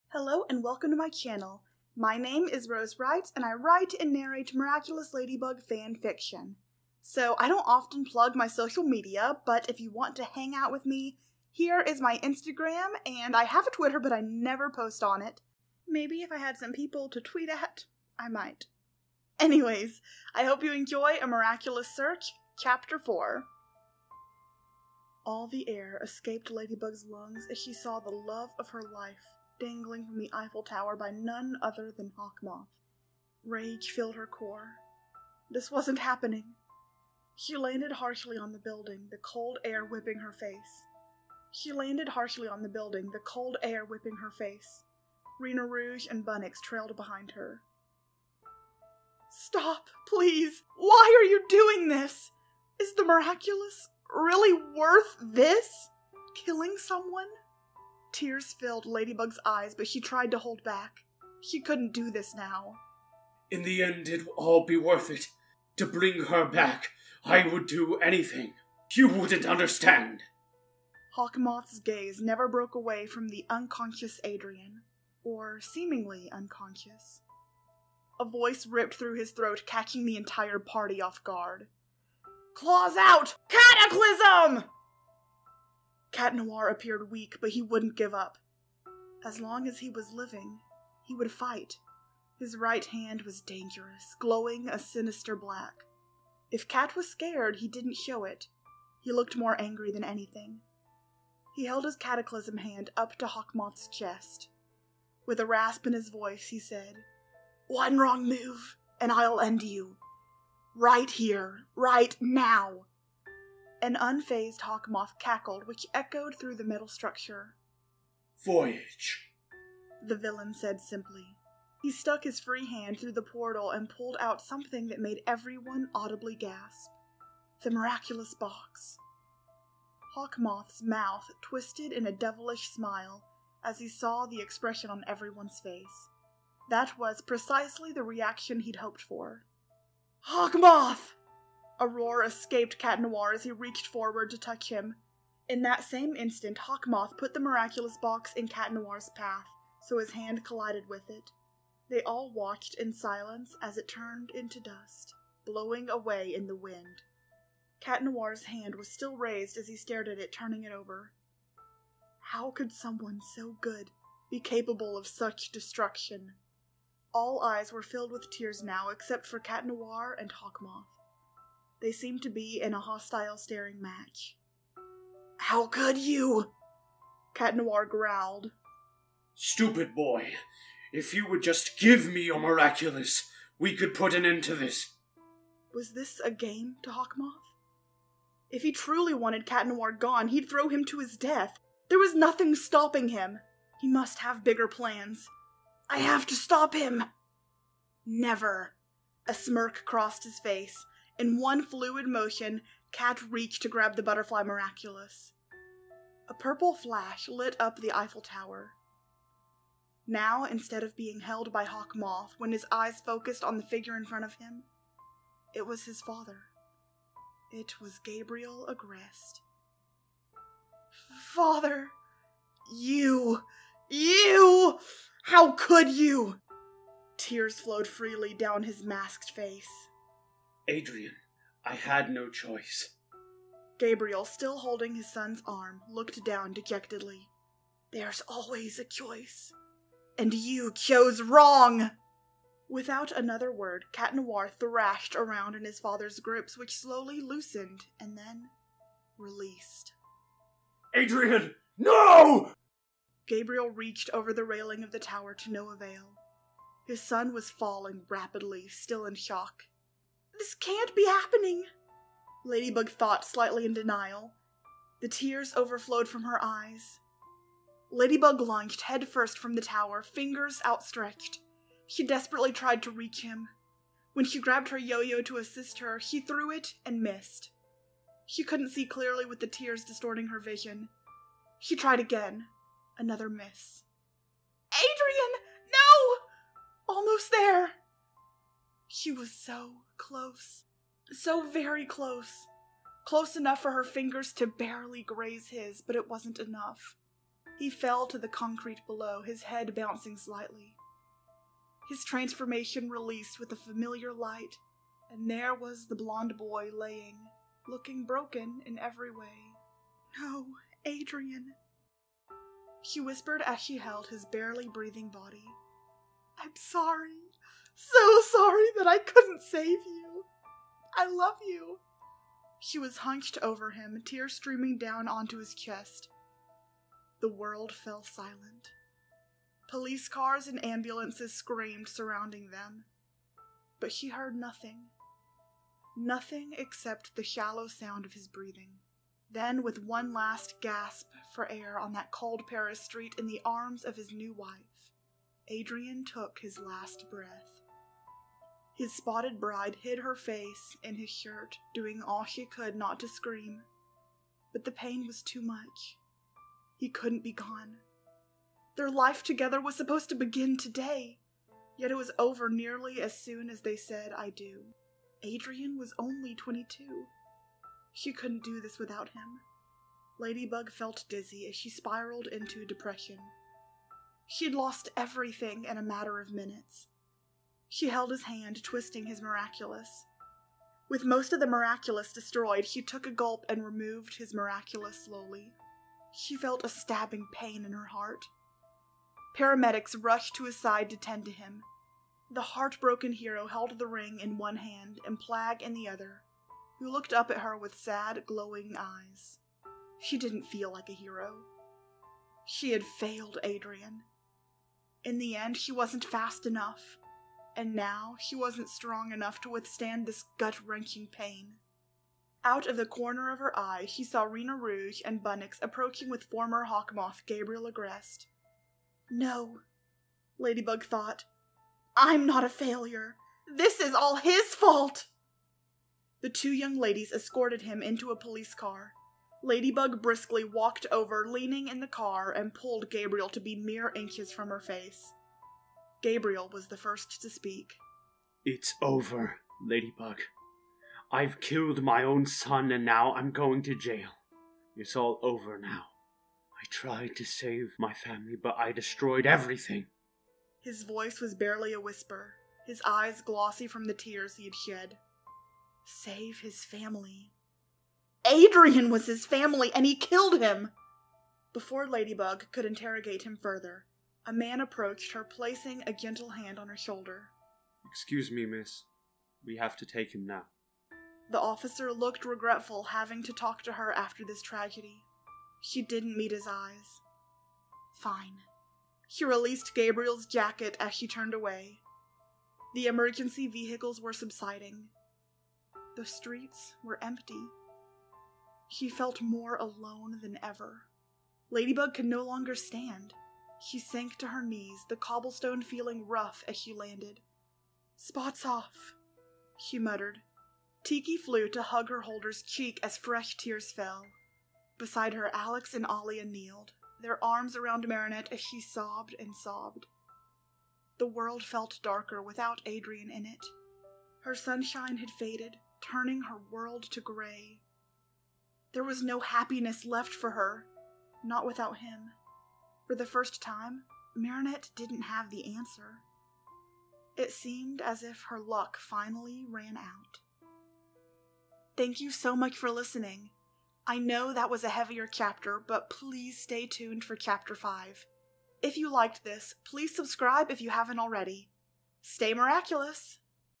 A Miraculous Search | Chapter 4 | (Darkfic, Death, Angst) | A Miraculous Ladybug Fanfiction
We're back atop the Eiffel Tower where Adrien/Cat Noir finds himself in the clutches of the evil Hawk Moth. After a startling reveal,  Cat Noir falls from the severe height. His only hope is Ladybug, but will she rescue him in time?

Chapter 4: You're here!
Chapter 5: Check back soon!

STAY MIRACULOUS! ♫